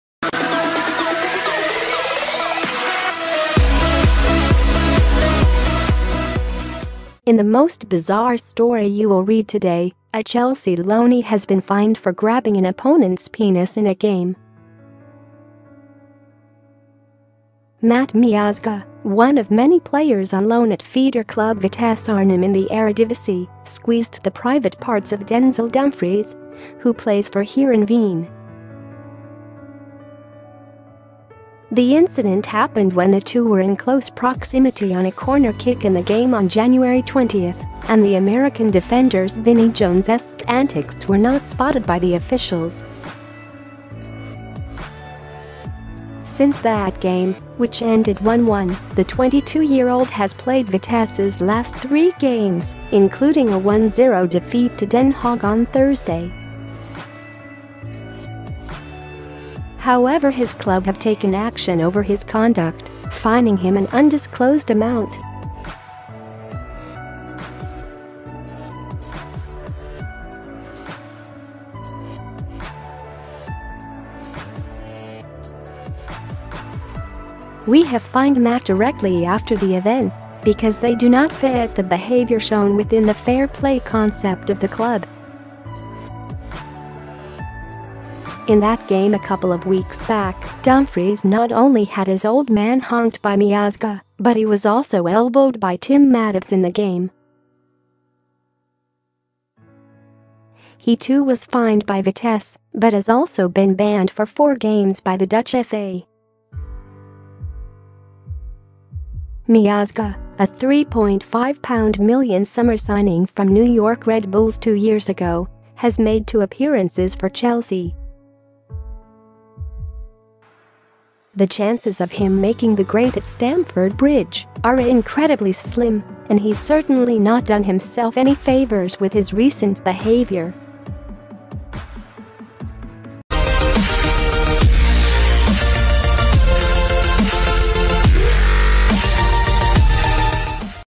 Chelsea Loanee Fined For Grabbing Opponent's Junk In Match
In the most bizarre story you will read today, a Chelsea loanee has been fined for grabbing an opponent's penis in a game. Matt Miazga, one of many players on loan at feeder club Vitesse Arnhem in the Eredivisie, squeezed the private parts of Denzel Dumfries, who plays for Heerenveen. The incident h .